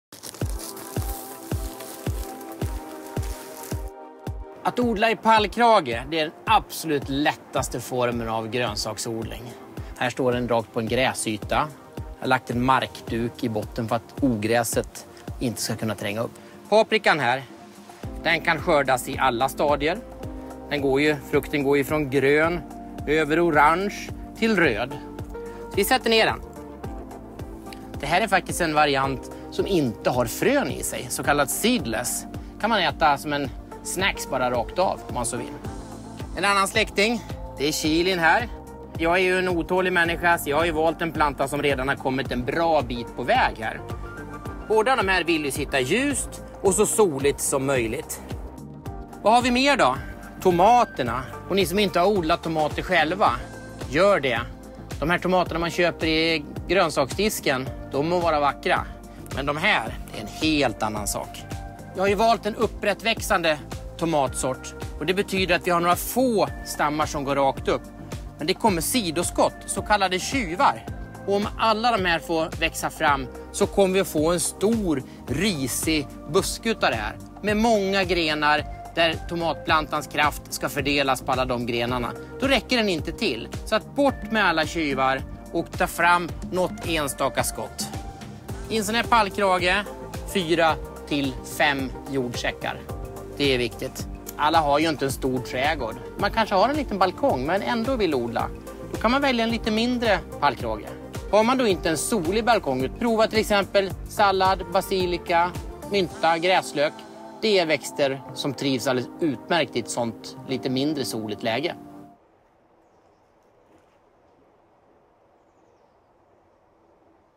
Odla i pallkrage - så gör du
Så lyckas du med att odla i pallkrage - Plantagens Johan Orre ger dig sina bästa tips.